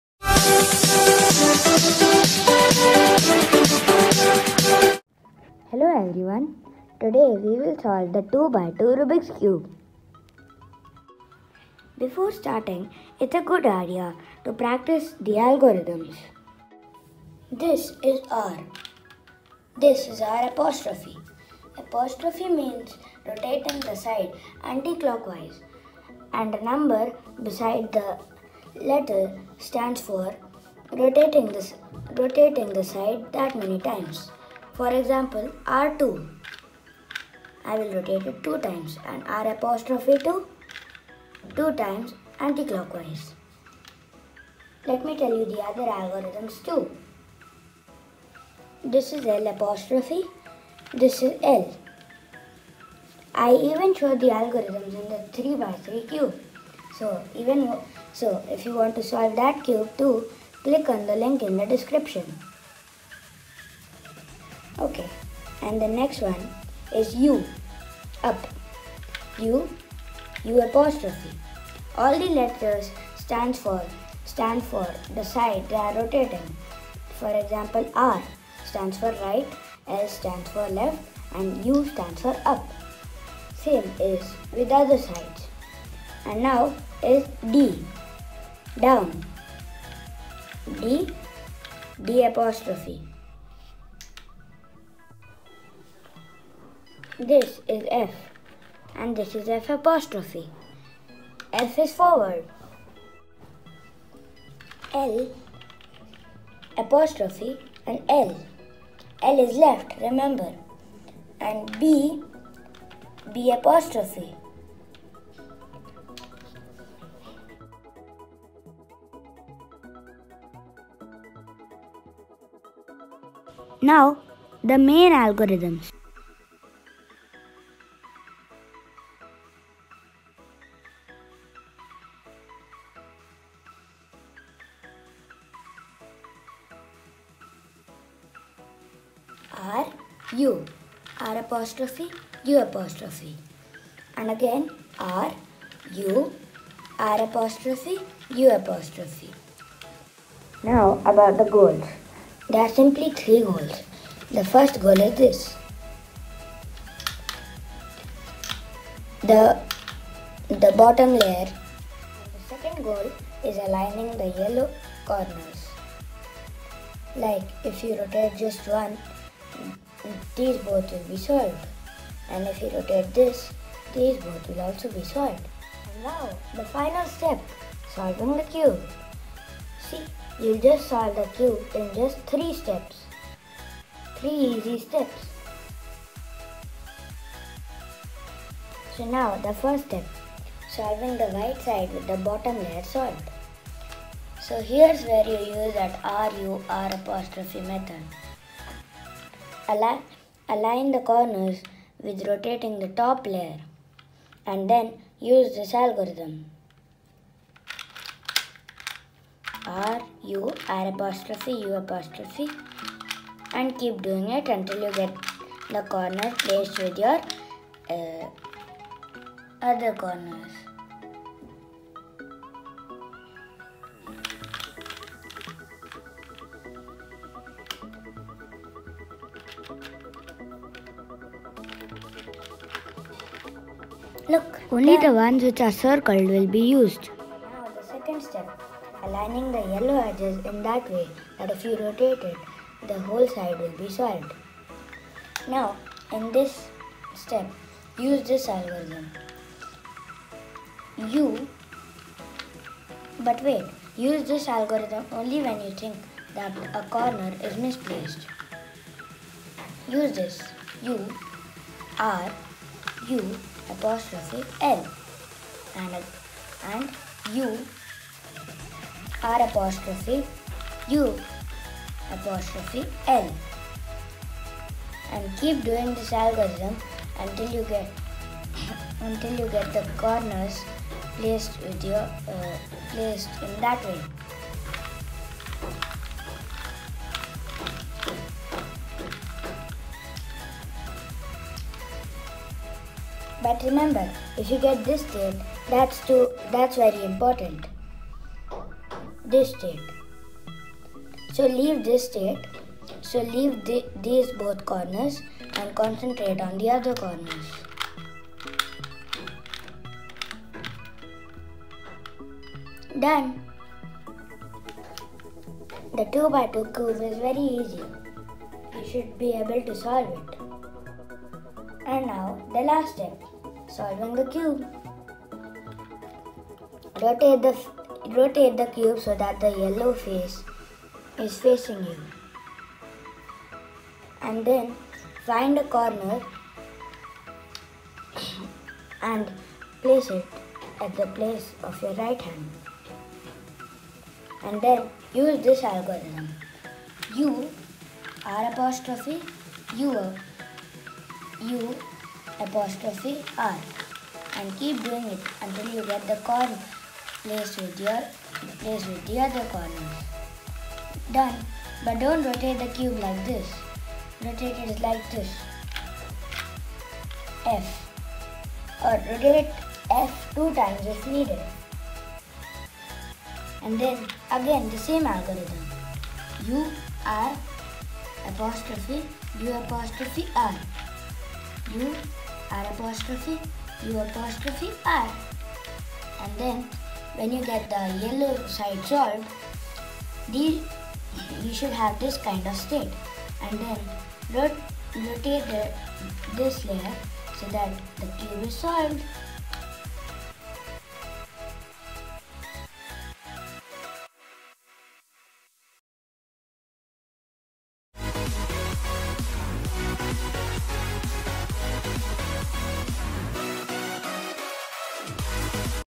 How to solve the 2x2 Rubik’s cube. Tutorial.
Hello everyone! Do u want to solve The 2x2 Rubik’s cube? Watch this!

LSS!

Timestamps: Solving the bottom layer: 3:08
                        Orienting the yellow corners: 3:57
                        Solving the yellow corners: 5:20